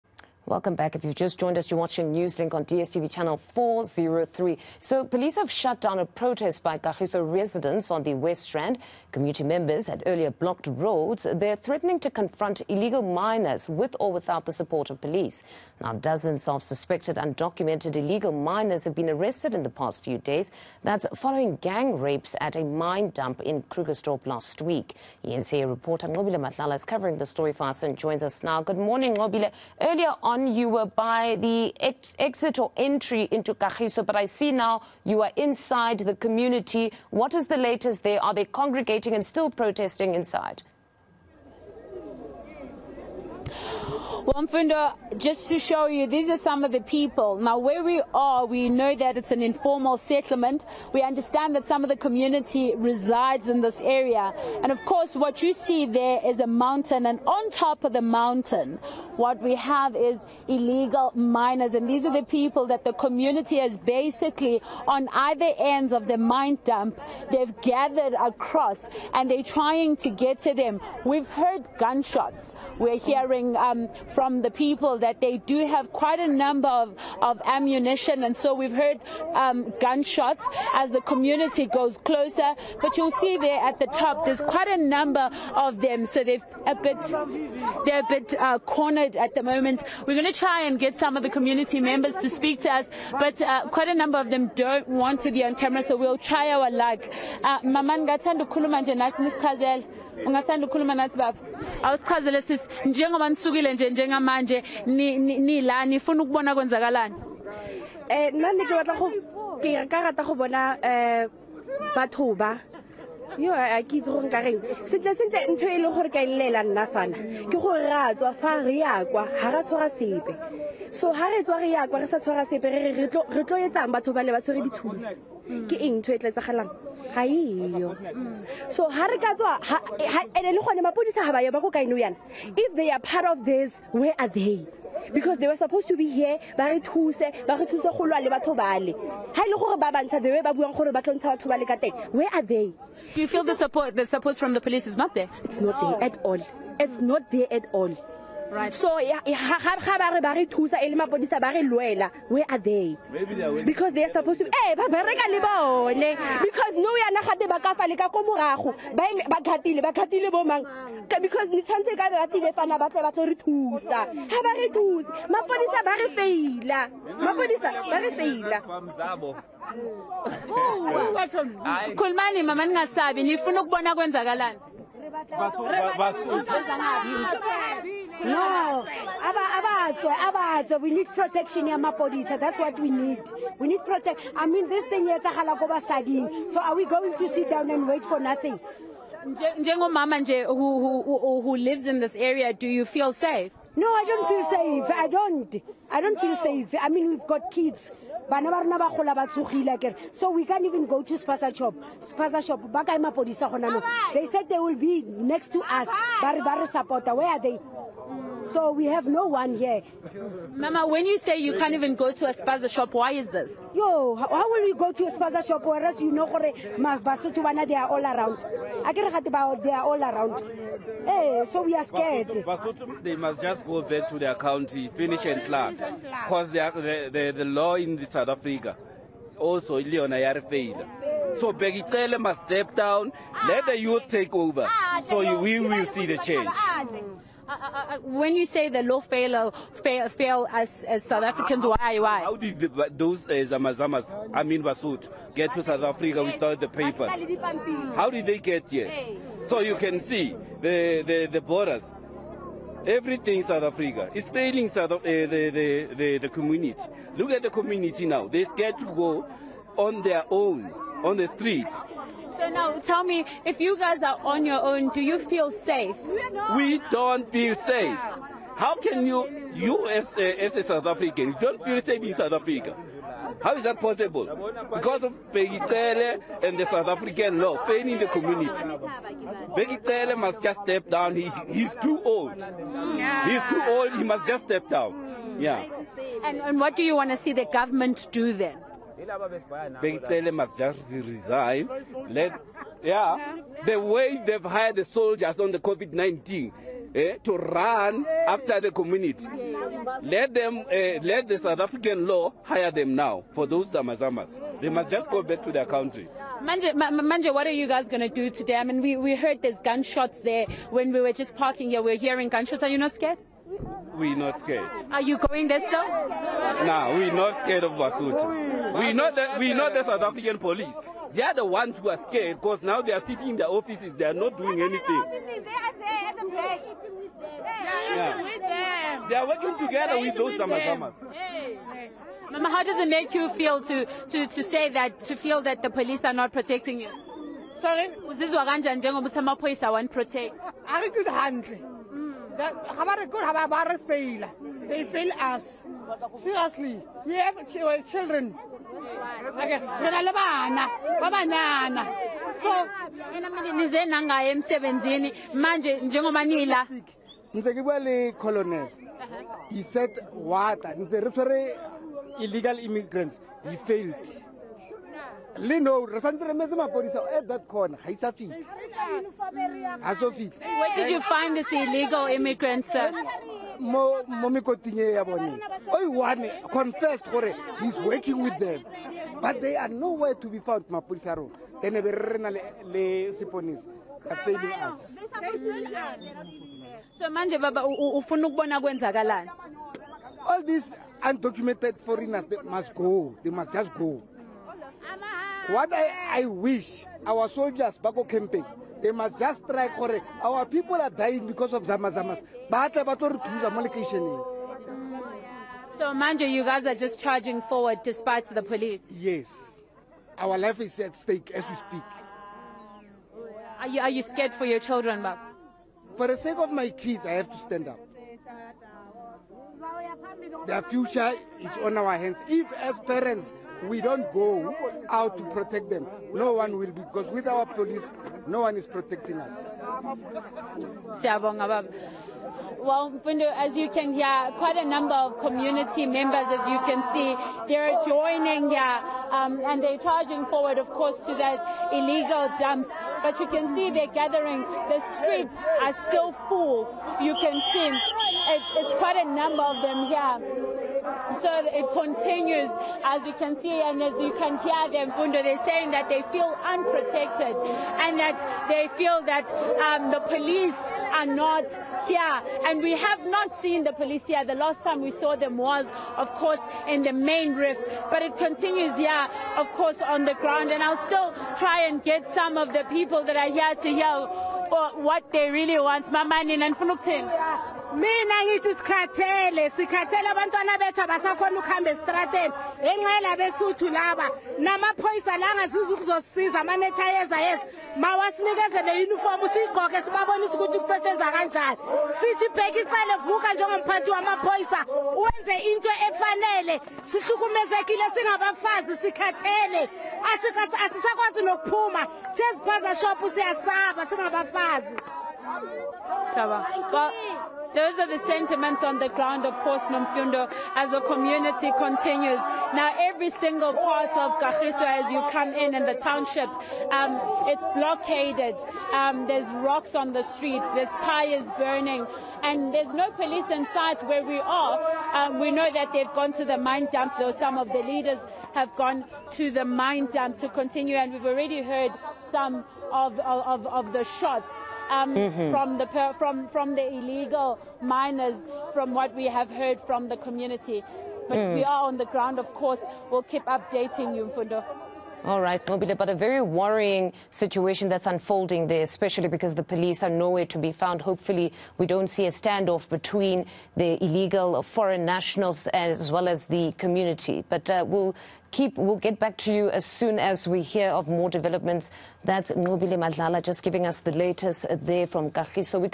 Kagiso residents protest against illegal mining
Communities on Gauteng's West Rand are protesting against illegal mining and the rising crime associated with it. They are threatening to confront illegal miners – with or without the support of police. eNCA's Nqobile Madlala has more details. Courtesy